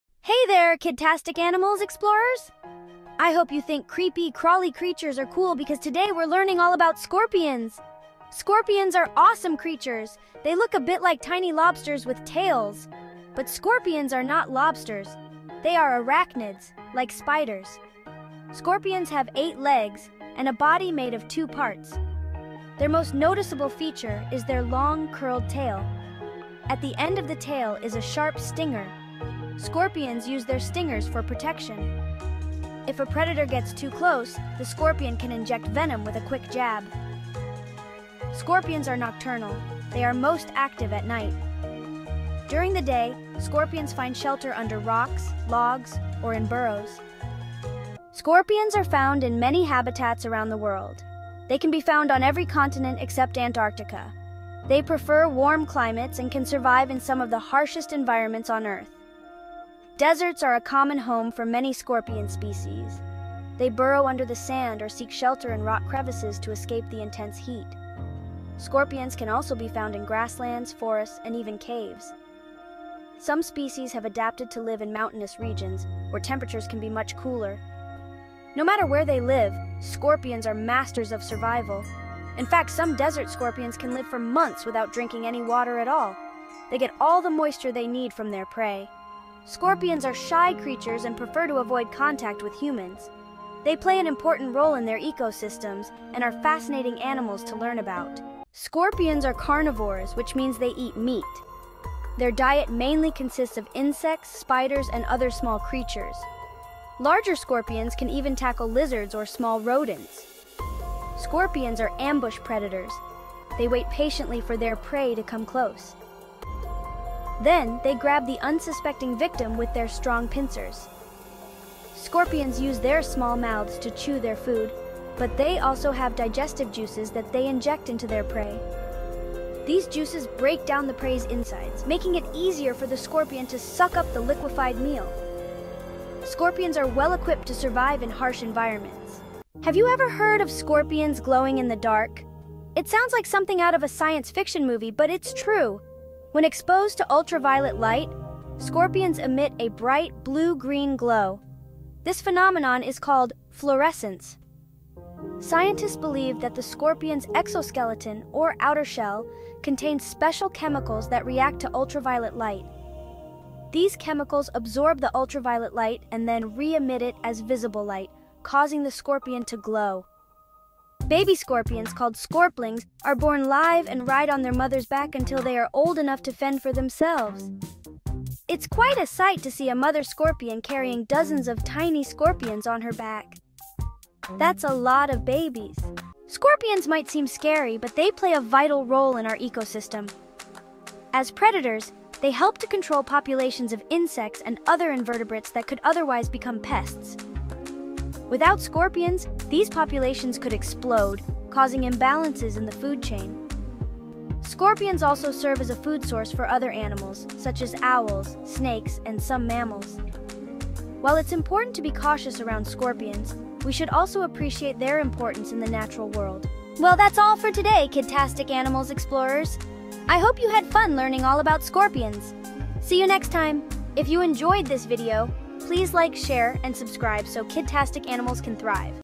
Scorpions: Arachnids That Glow! Animals for Kids
Discover the fascinating world of scorpions in our latest video! Learn how these incredible arachnids, with eight legs and a venomous sting, thrive in diverse habitats from deserts to forests.
Uncover their nocturnal behavior, unique survival tactics, and impressive water conservation skills.
Watch as we delve into their diet, hunting methods, and the intriguing fluorescence that makes them glow under ultraviolet light.
Find out how scorpions contribute to ecosystem balance by controlling insect populations and serving as prey for other animals. Join us for an educational journey across the globe, exploring over 2,500 species of these resilient creatures.

Chapters
0:00 Meet the Scorpions!
0:47 Where do Scorpions Live? ￼
1:38 What Do Scorpions Eat?
2:19 Arachnids That Glow!
2:50 Scorplings
3:06 Why We Need Scorpions
3:36 Thanks for Watching!